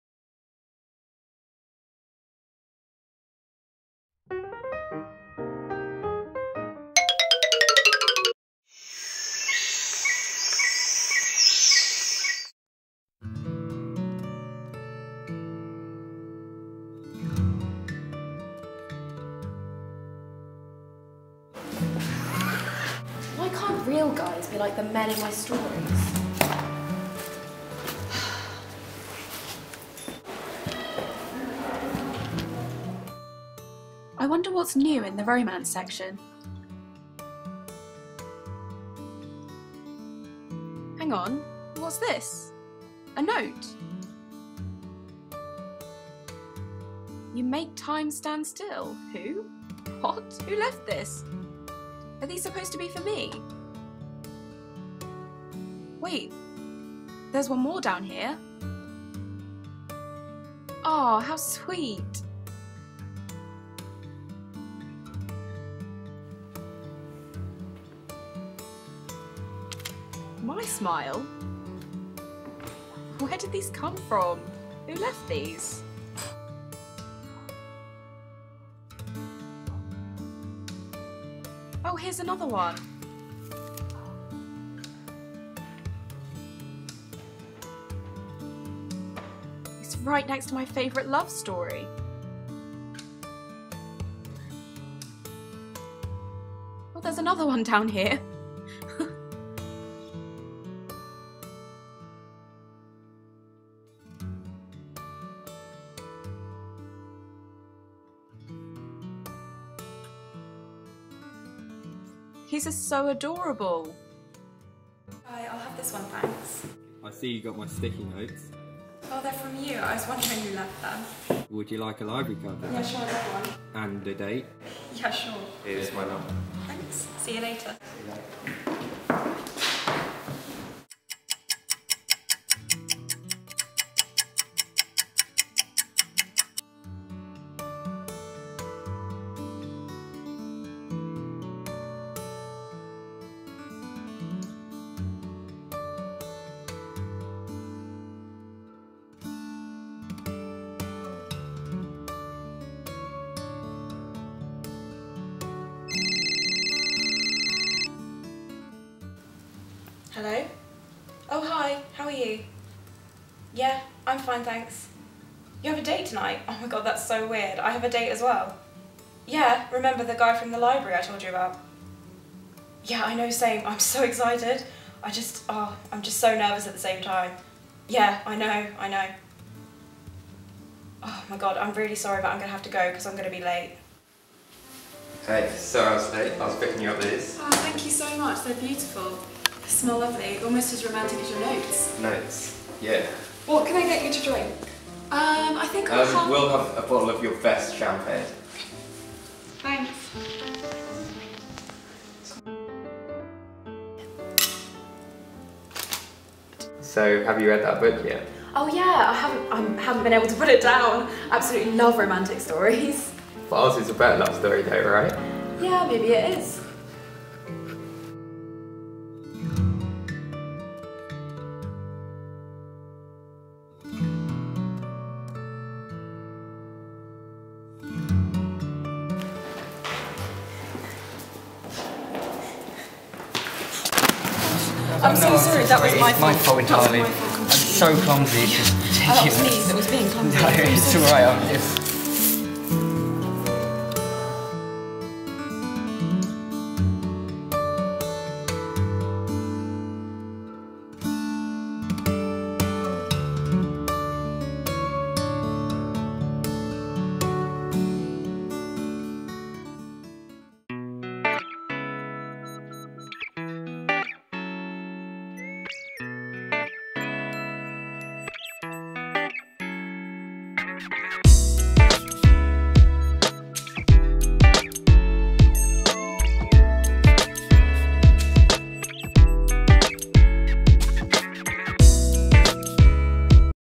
Checkout Short Film
A romantic short film created for A2 Media Studies.